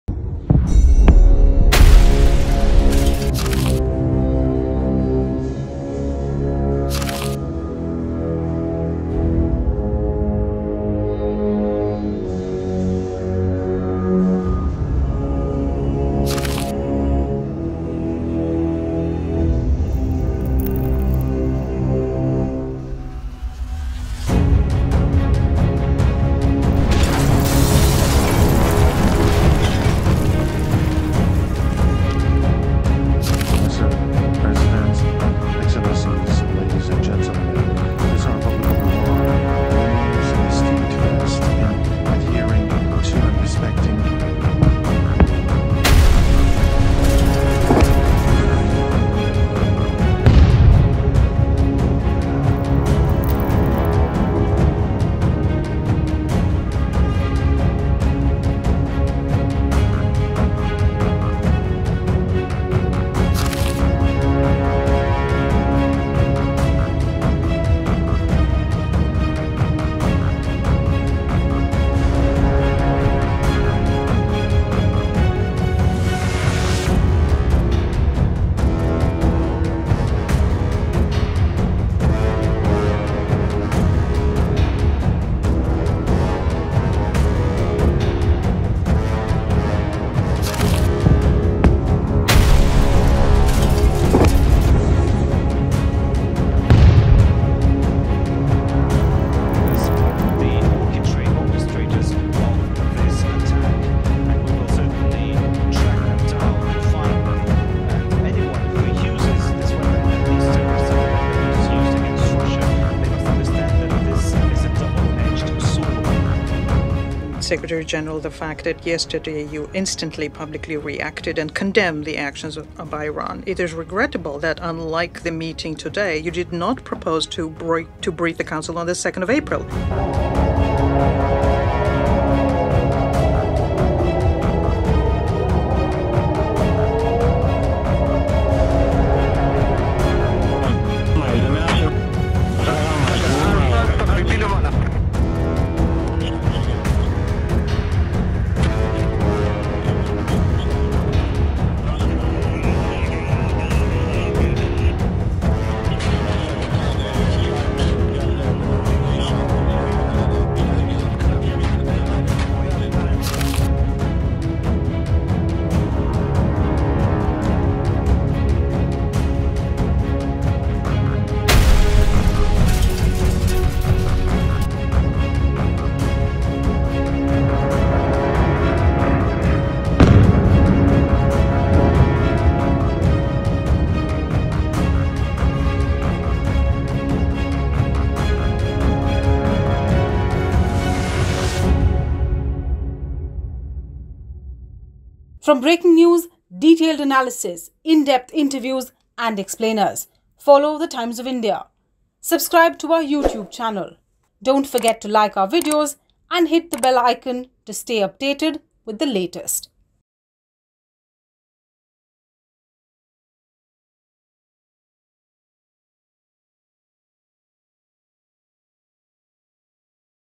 Putin Sides With Iran: Lavrov Dials Up Iranian Counterpart, Warns Israel Again | Details
In the latest developments, Iran's Foreign Minister reiterated Tehran's actions as a warning and punitive measure against Israel. Meanwhile, Russia has condemned the attack on Iran's consulate in Syria and criticized the UN Security Council for overlooking it. Amid escalating tensions, Iran launched a barrage of missiles at Israel in retaliation for the bombing of its Syria consulate, with Israel claiming to have intercepted 99% of them. The global community, including the UK, Germany, France, and others, has urged Israel to exercise restraint to prevent further escalation. Click to Find out more.